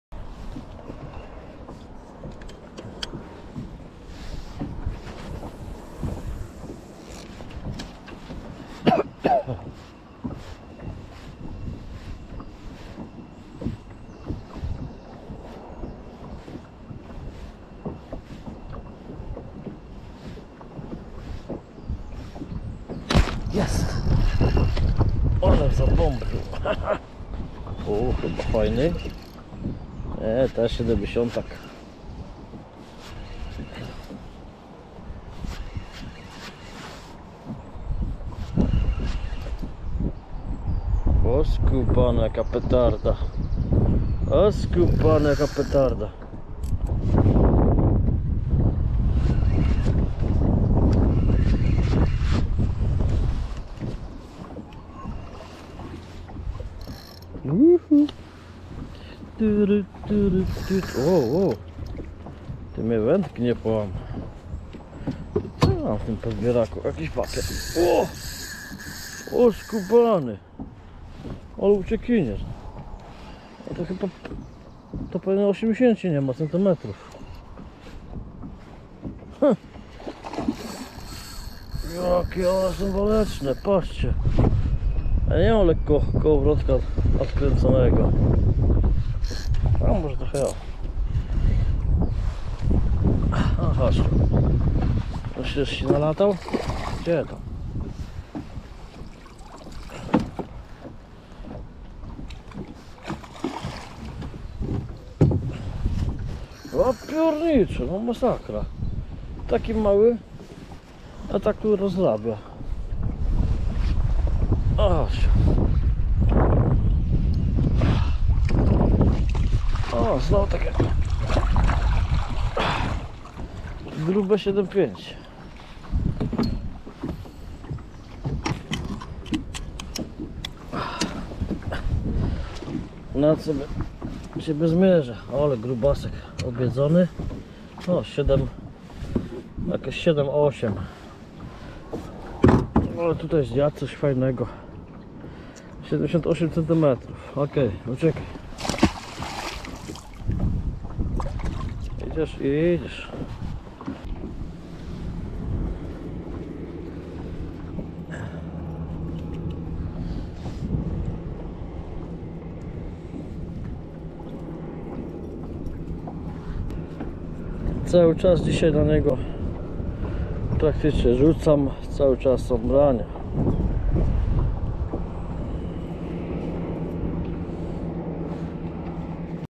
Potezne branie i walka z silnym szczupakiem
Zapraszam na kolejny pelen emocji hol szczupaka ;)